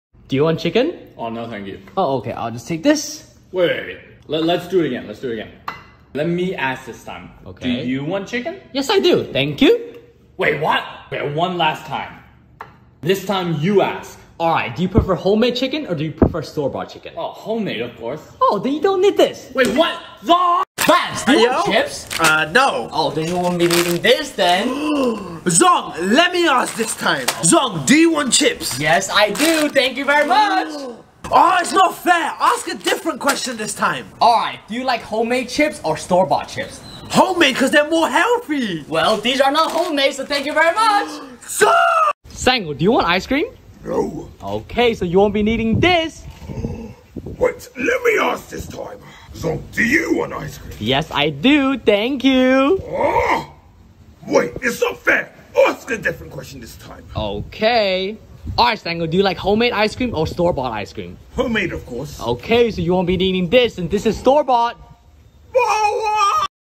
Zhong took 3 friends food
Zhong trick on Nich , bants & Sango.